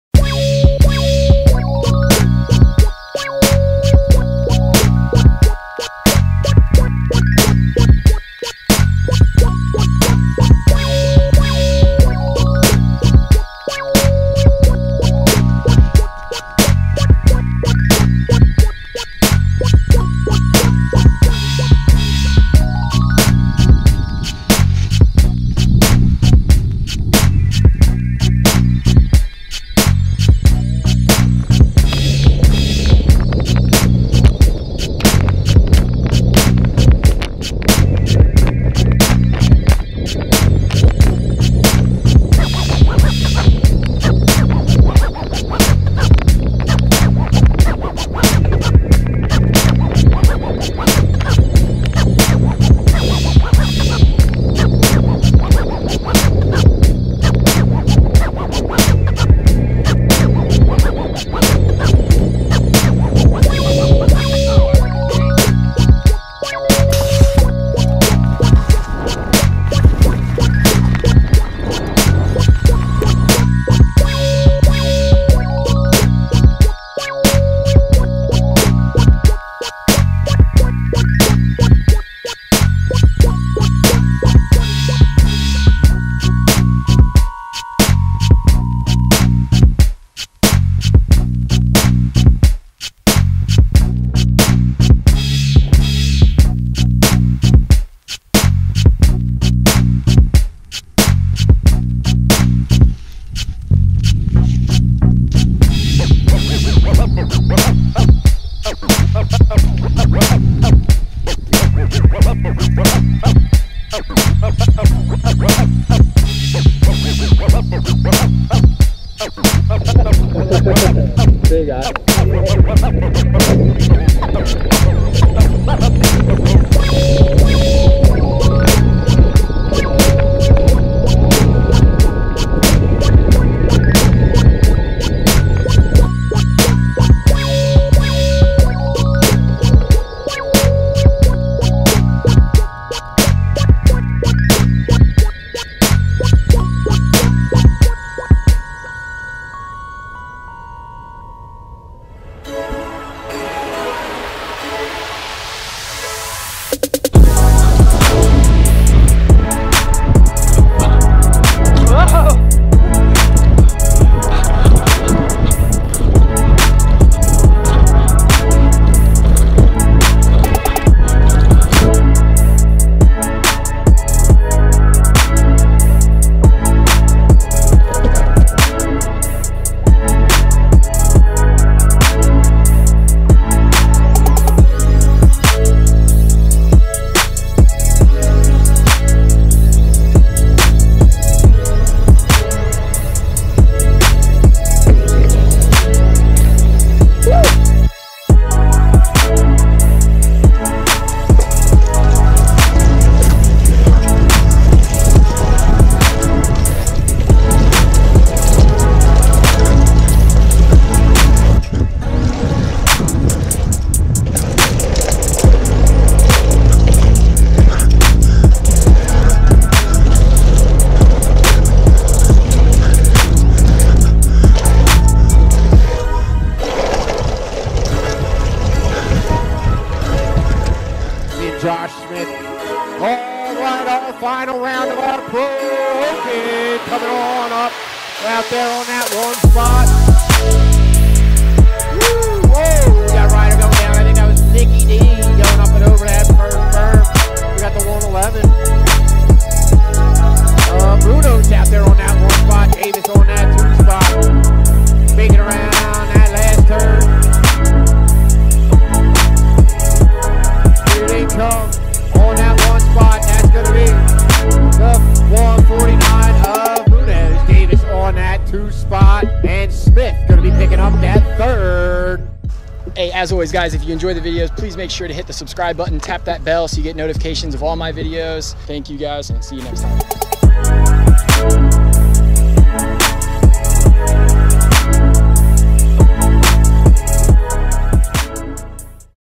BMX Race: 2018 Recap
Here's a short look back on some of the BMX and Downhill Mountain Bike racing and riding I did throughout the 2018 season. I raced a bit and rode a lot! I'm looking forward to 2019 with a ton of BMX RACING!

Make sure to check out the riding here from Dayton Indoor BMX, Mike's Bike Park, Lebanon Bike Park Pump Track, Toledo BMX Track, Snowshoe Mountain Bike Park, and Beech Mountain Bike Park!

If you've never rode a BMX bike (or just want to get better at it), check out my HOW TO RIDE playlist

Category
Sports